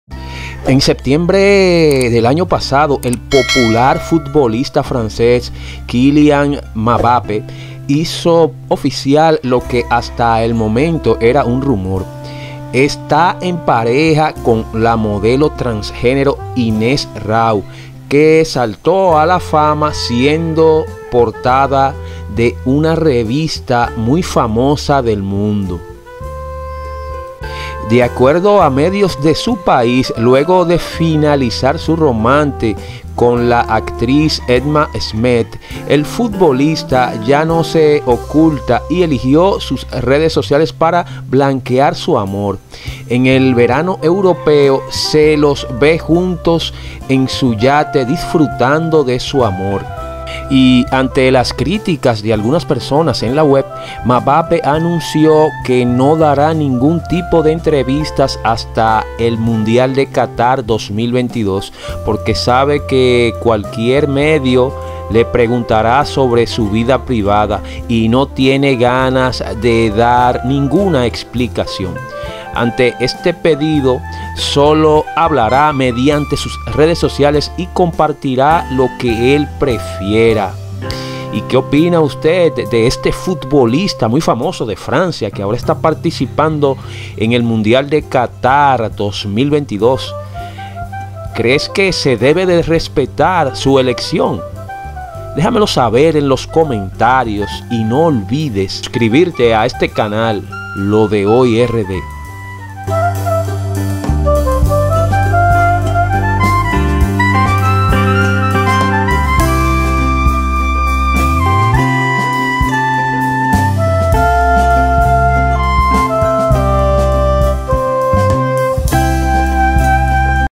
Famoso futbolista deja su novia por una transgénero, Mundial de Qatar 2022
Sigue nuestra pagina: LodeHoyRD.com